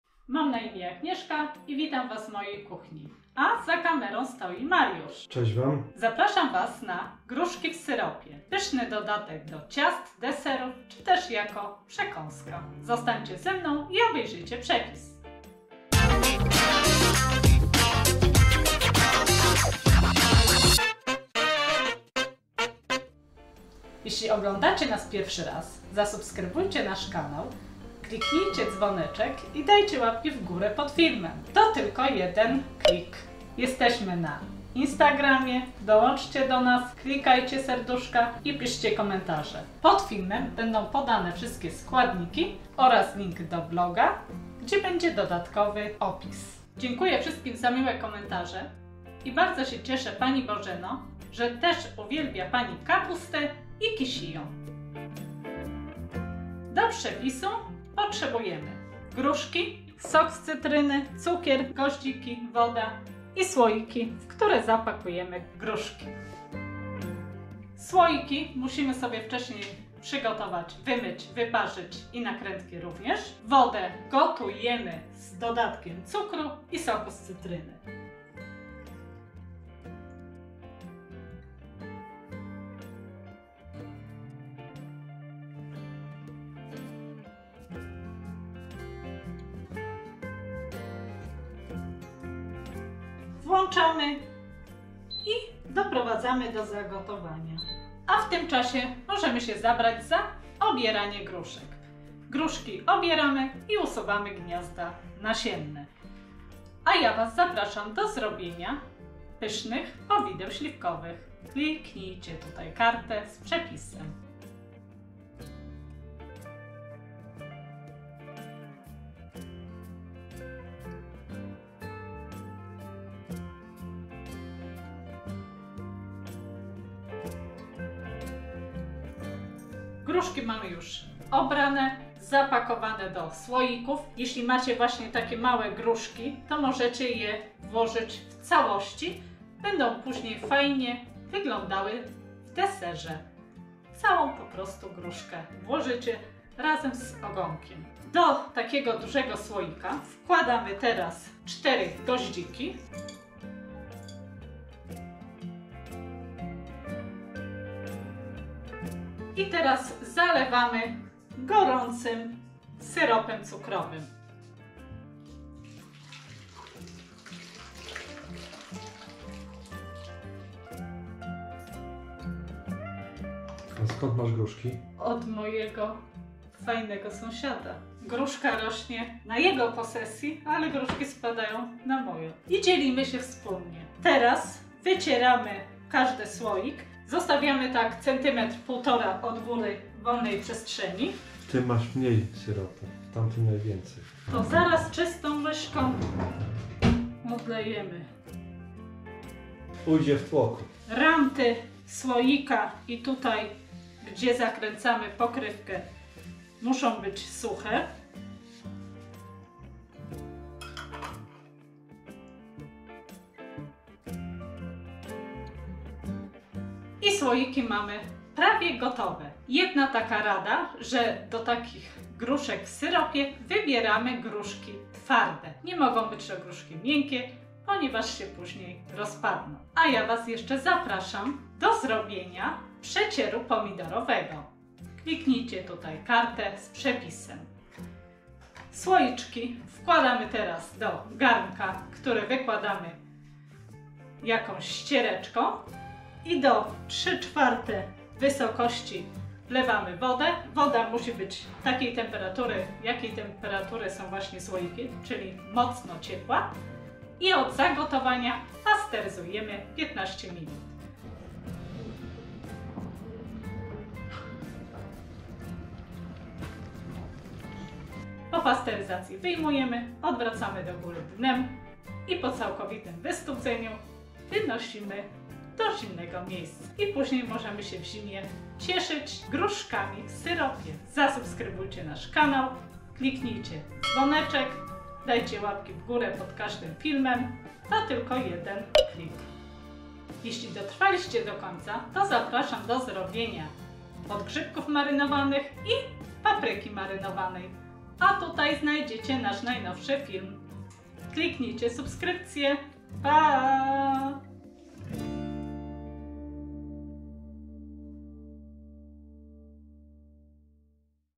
Przepis na gruszki w syropie w słoikach - Przetwory na zimę -371
Gruszki w syropie w słoikach, to idealna forma do przechowania na czas zimy. W odpowiednim momencie można wyciągnąć i zastosować jako dodatek do dania, zrobić deser lub co tam nam przyjdzie do głowy.
👉Jak zrobić gruszki w syropie w słoikach na zimę❓
Przygotowanie takich przetworów jest po pierwsze bardzo proste i szybkie. Wystarczy zrobić zalewę cukrową, obrać gruszki i zamknąć smak lata w słoikach...
Zapraszamy na przepis na gruszki w słoikach.

Mariusz i Agnieszka

Spis
0:00 - 0:54 - Zapraszamy na przepis na gruszki w syropie
0:55 - 1:06 - SKŁADNIKI potrzebne do zrobienia gruszek w syropie
1:07 - 1:31 - Doprawiamy wodę na syrop
1:32 - 2:08 - Gotujemy wodę i w tym czasie obieramy gruszki
2:09 - 2:37 - Kilka słów o gruszkach z ogonkiem i goździki
2:38 - 3:02 - Zalewamy gorącym syropem cukrowym
3:03 - 3:56 - Wycieramy brzegi do sucha
3:57 - 4:27 - Jak pasteryzować?
4:28 - 5:12 - Co robimy po pasteryzacji

Składniki:
📌    2kg gruszek (muszą być twarde)
📌    1,5 litra wody
📌    500g cukru
📌    sok z połowy małej cytryny
📌    goździki (po 4 sztuki na każdy duży słoik 0,9l)